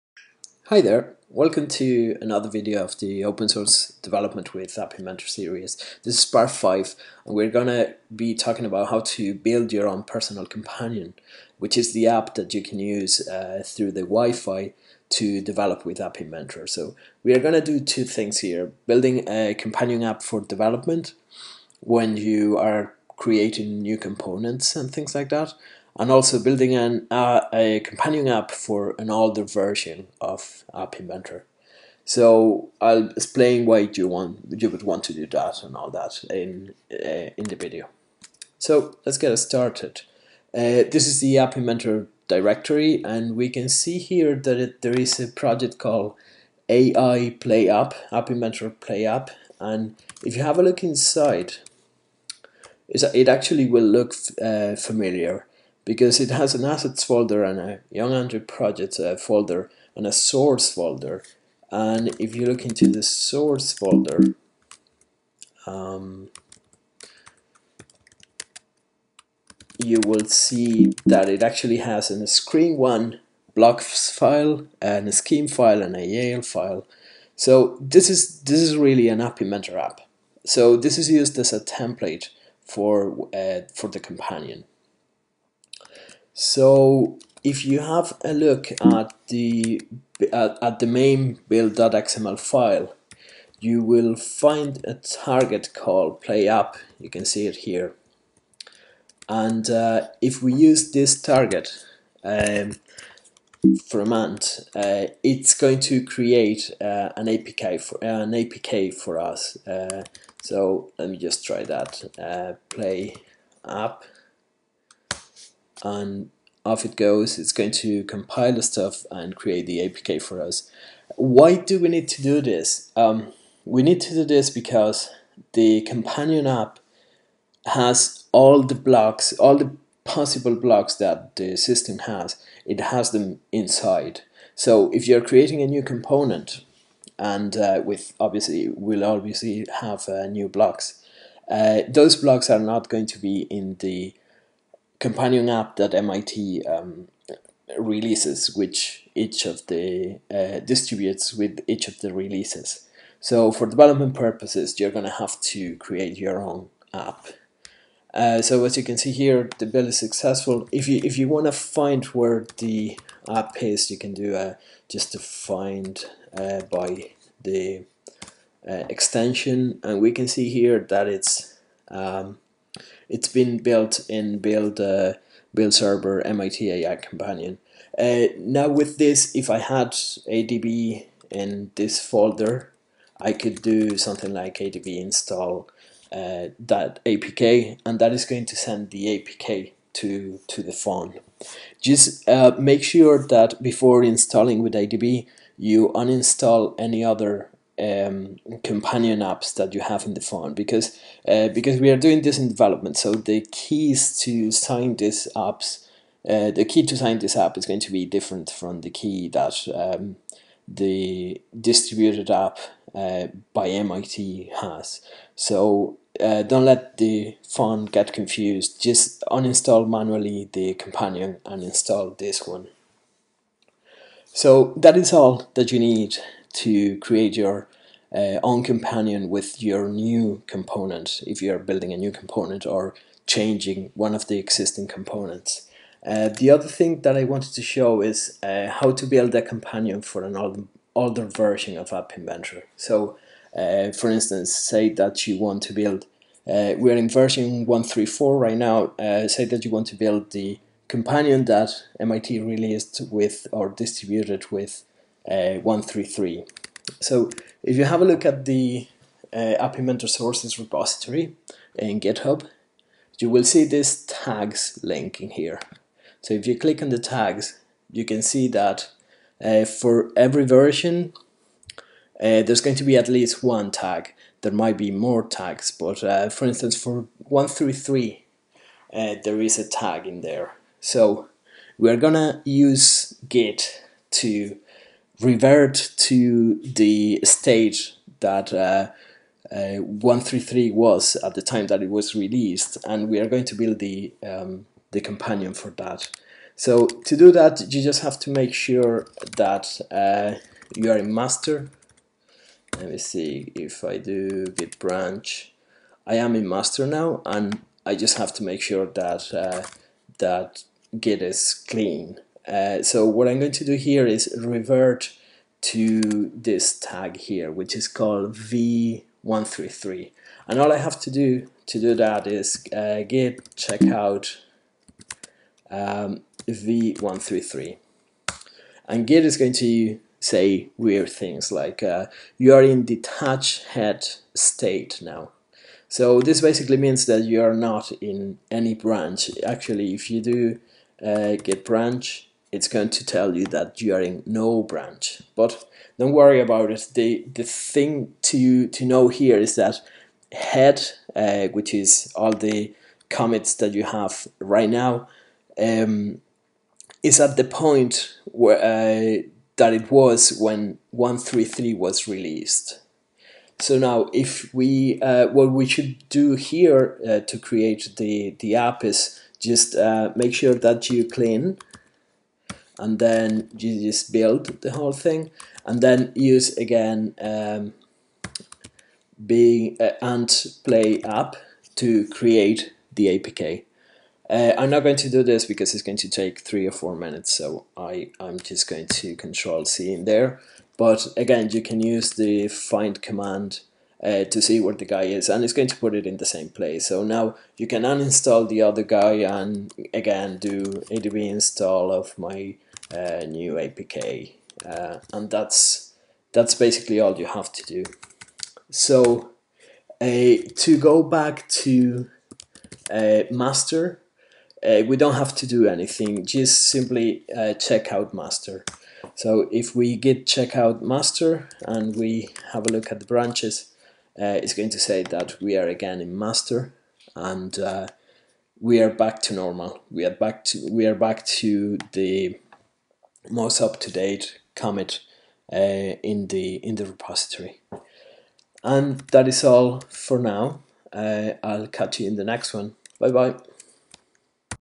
Open Source Development with App Inventor - Part 5 : Your own Personal Companion
This is part 5 of the Open Source Development with App Inventor series. In this video we will see how to package a Companion app while we are developing a new component, or if we need one for an older version of App Inventor